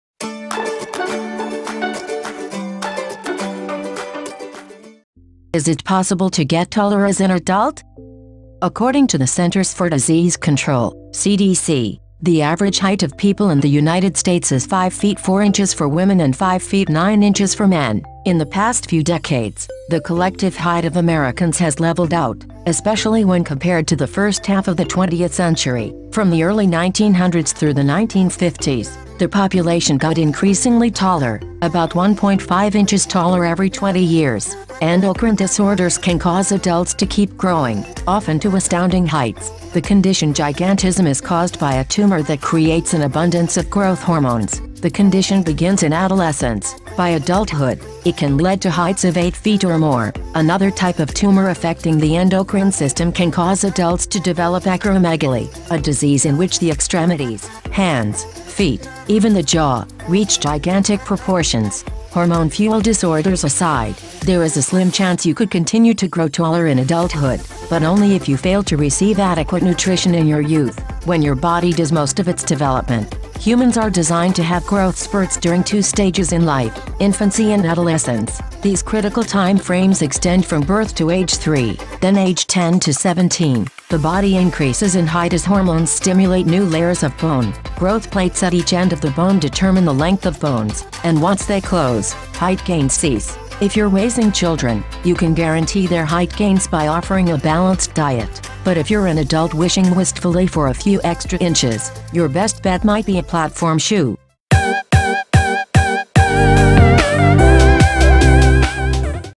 Is It Possible to Get Taller as An Adult?
According to the Centers for Disease Control (CDC), the average height of people in the United States is 5 feet 4 inches for women and 5 feet 9 inches for men. In the past few decades, the collective height of Americans has leveled out, especially when compared to the first half of the 20th century. From the early 1900s through the 1950s, the population got increasingly taller — about 1.5 inches taller every 20 years